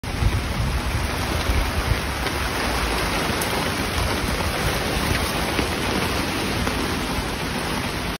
Out in the Rain1
On site today ☔️ ☔️ ☔️ Anyone else out in this? 🌨️

Were on site connecting a new generator. Their old generator was outgrown by the expanding operations and demand for power ⚡️ We had to retire the old bumblebee 🐝 🤖 and bring in the replacement.

But as you know... no job is simple... look out for what we had to do inside the building 🦾 🧰 🔧